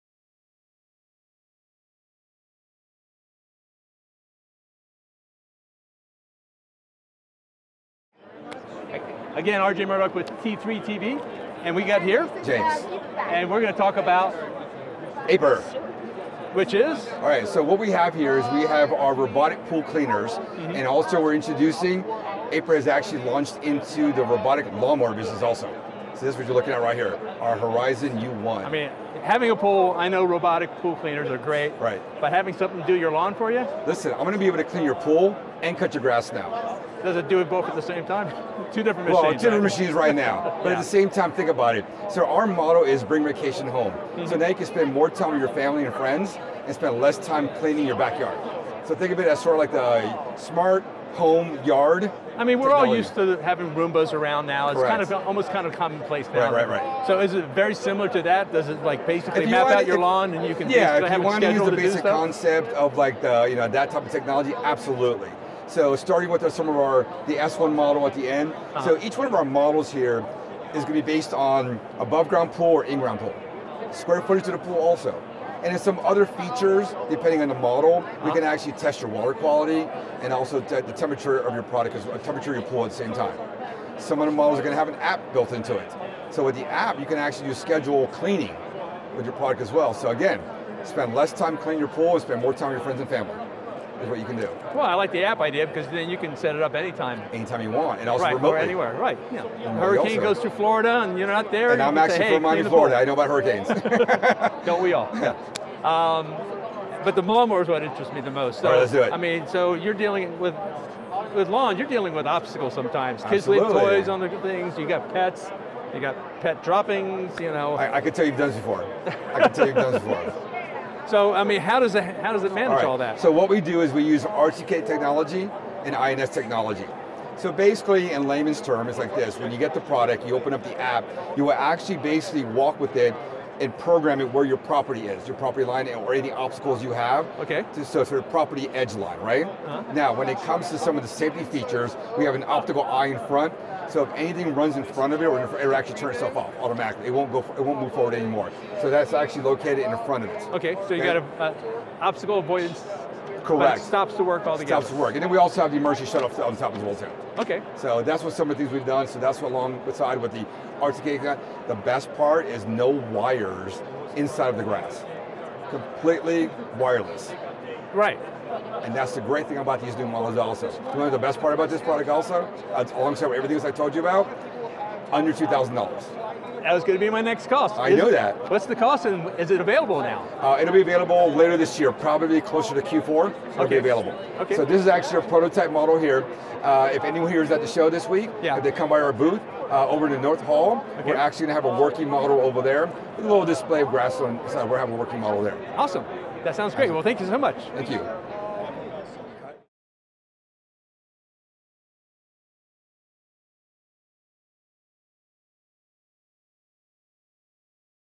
CES 2024 Unveiled - Aiper Robotic Lawn Mower (8K 3D VR180)
Check out the latest and coolest cordless lawn mower, the Aiper Horizon U1, as shown in 3D180 from the CES 2024 Unveiled media event on January 7, 2024.

8K 3D VR180 footage shot by RJ Murdock with T3-TV using his Canon R5C with Dual 5.2mm lens. It is best watched on a VR headset.  If on a 2D computer monitor, click-hold and drag  your mouse around to view the entire scene.